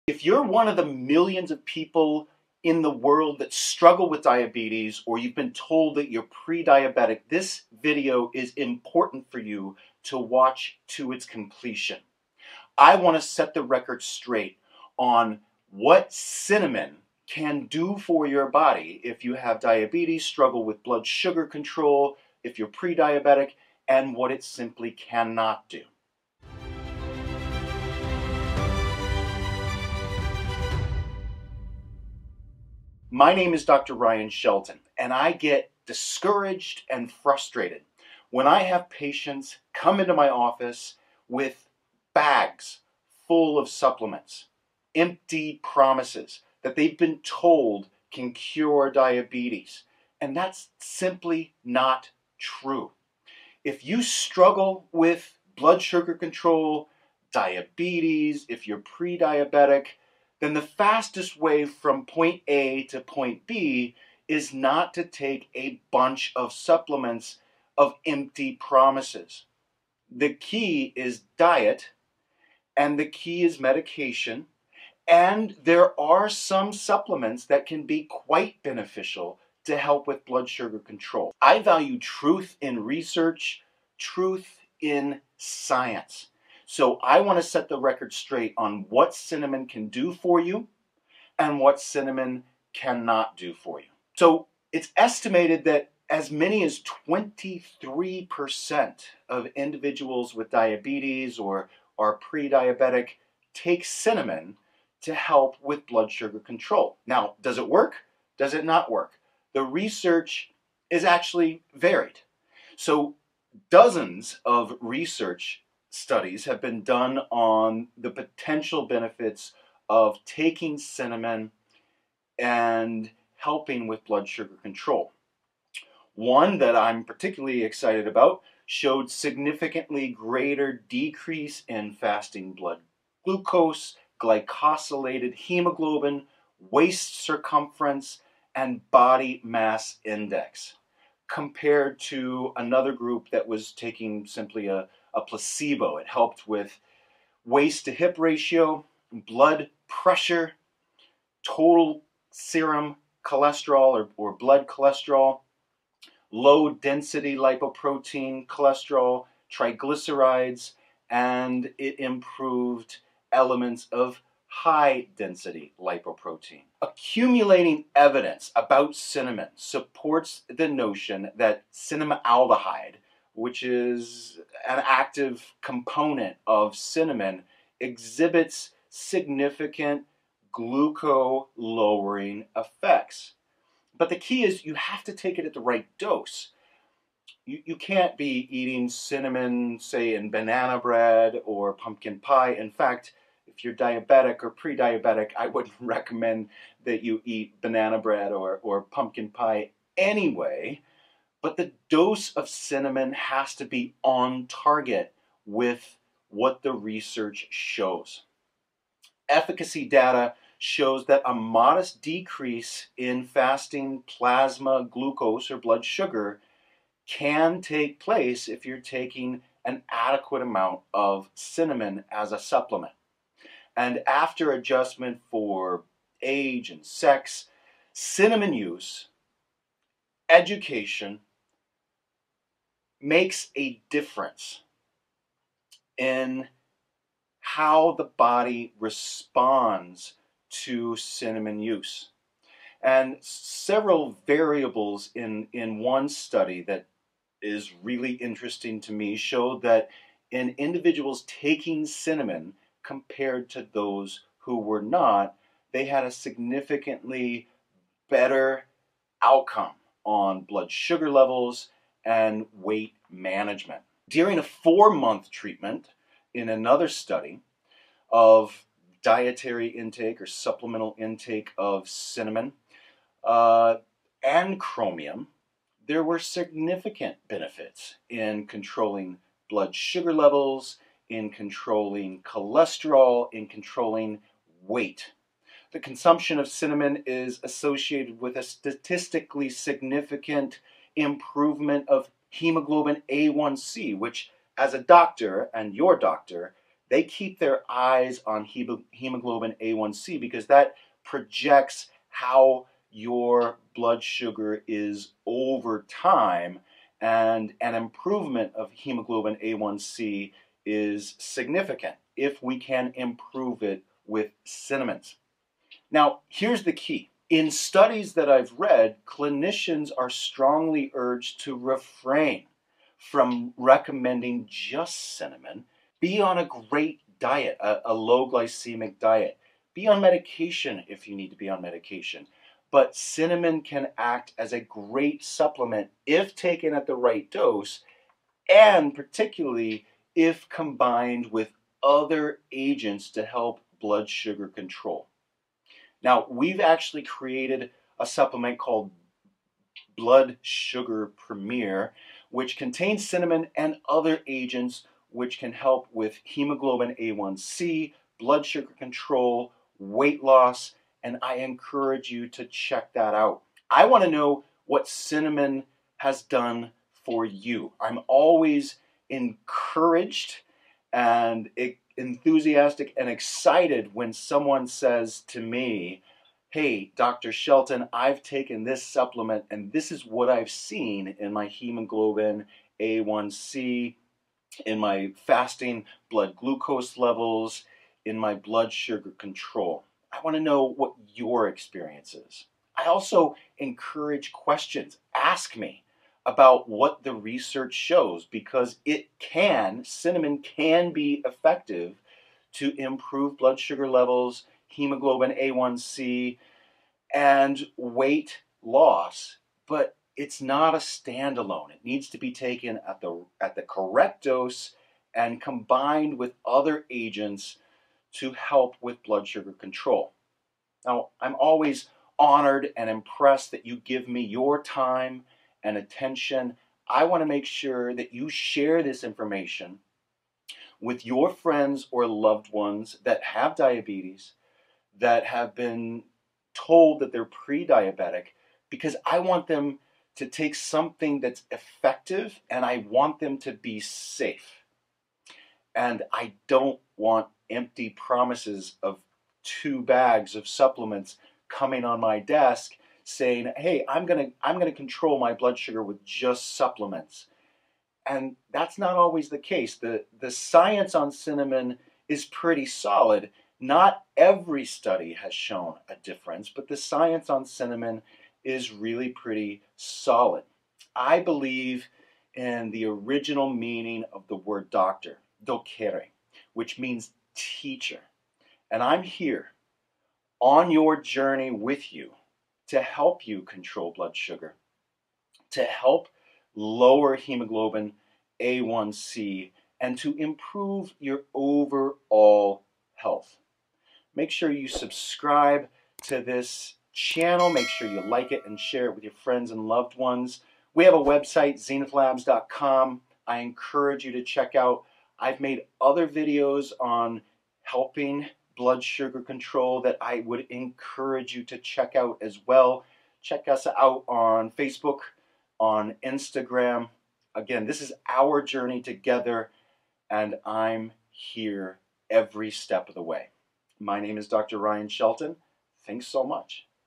What Cinnamon Does To Your Body If You Have Type 2 Diabetes
In this video Dr. Ryan Shelton sets the record straight on what cinnamon can (and cannot) do for your body if you have diabetes, struggle with blood sugar control or if you're pre-diabetic.

Have You Seen Dr. Shelton's innovative supplement lineup to increase your health?